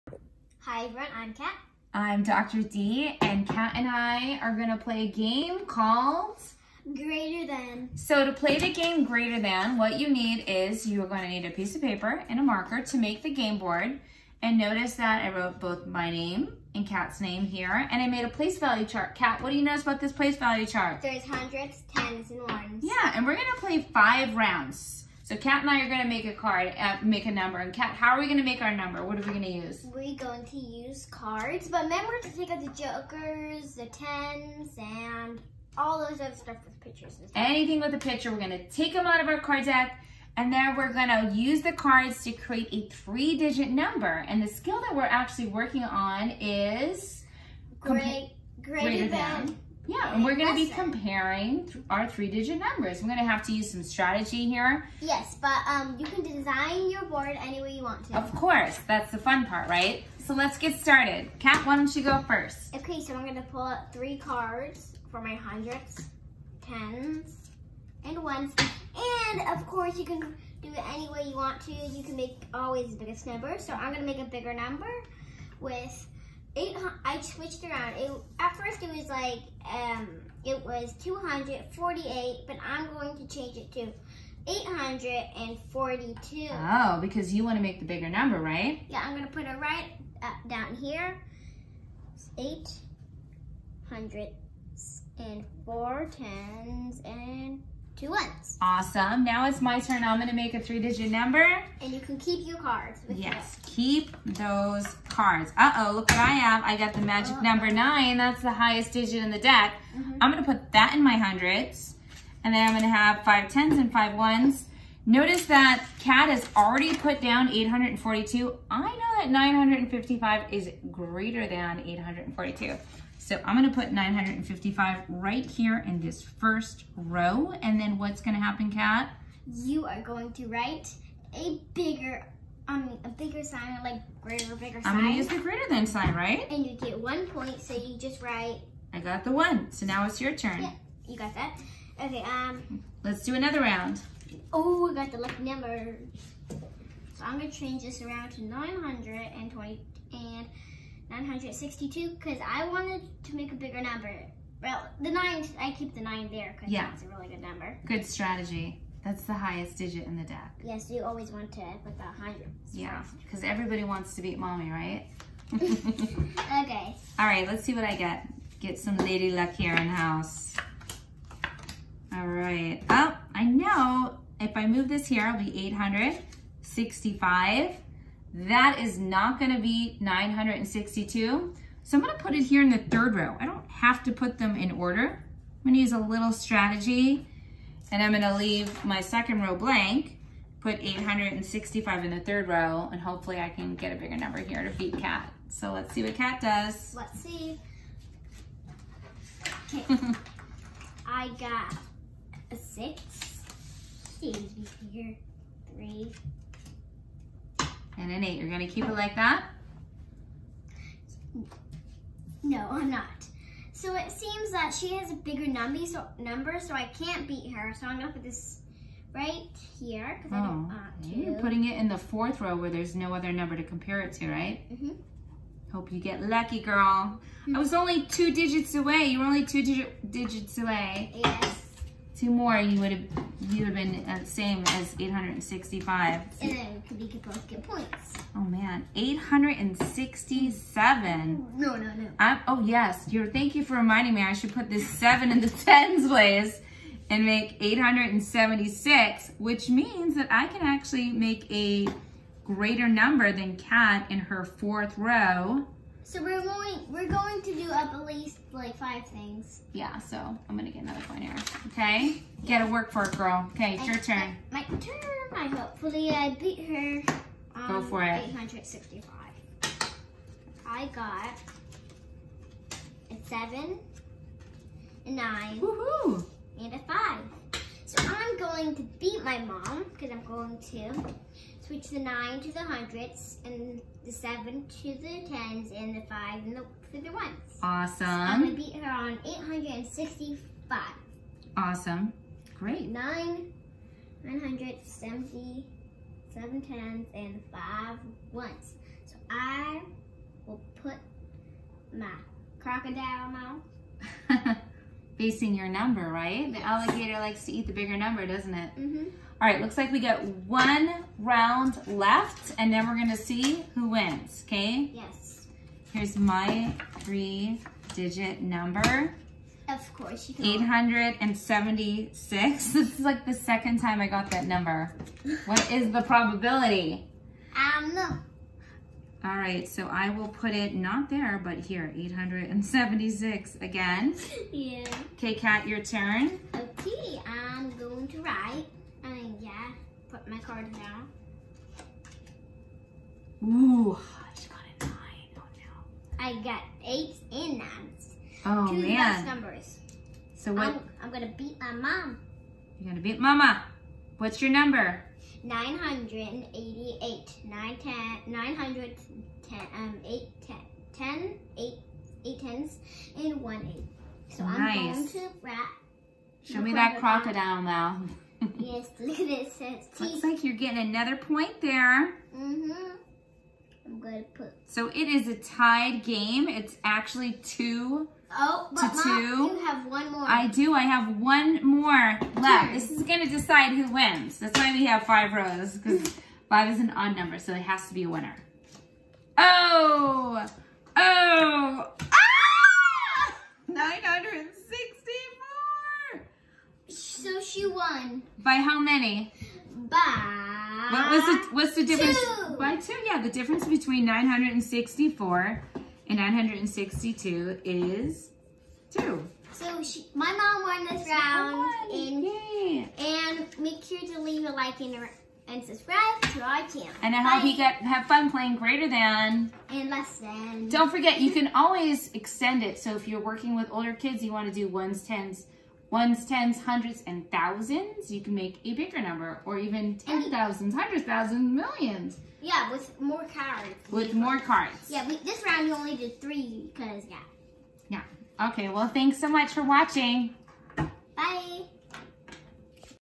FUN Math Game: Greater Than Comparing Three Digit Numbers/ Hundreds Tens and Ones: K-12 Math Summer
This game supports creating and comparing three digit numbers to reinforce place value skills and decomposing numbers. All you need is a deck of cards, paper and pen to create and compare three digit numbers.  The person with the greater number wins. Use the greater than and less than sign to compare.